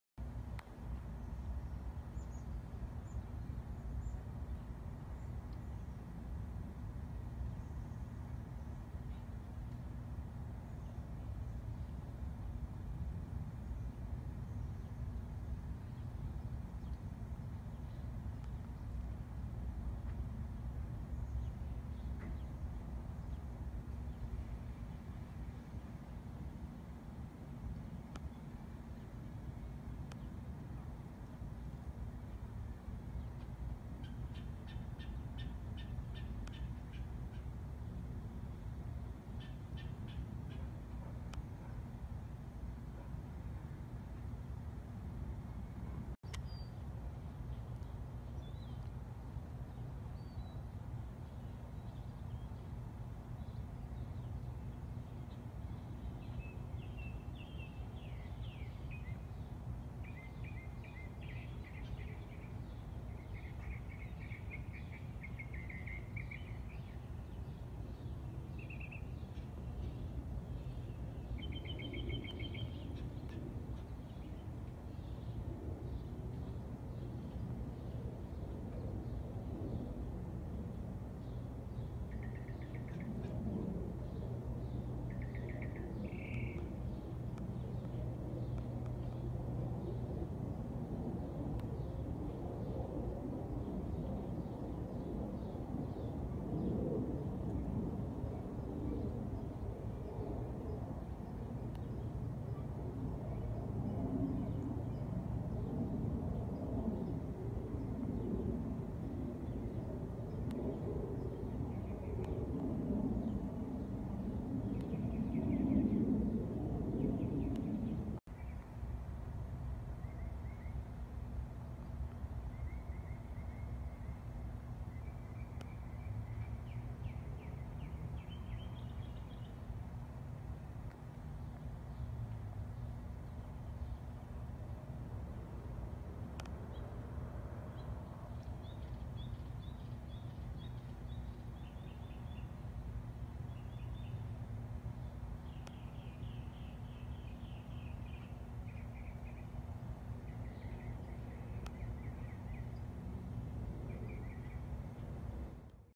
A Turkey Vulture Perched on a light pole at Rose Elementary, Milpitas, CA
I was standing a bit far, but zoomed to the max to get a good close up.